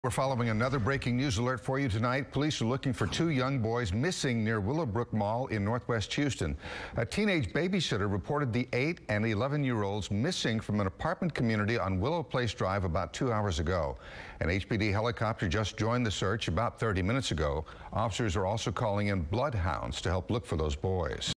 Search for missing boys
Houston police are searching for two missing boys Monday night in northwest Houston.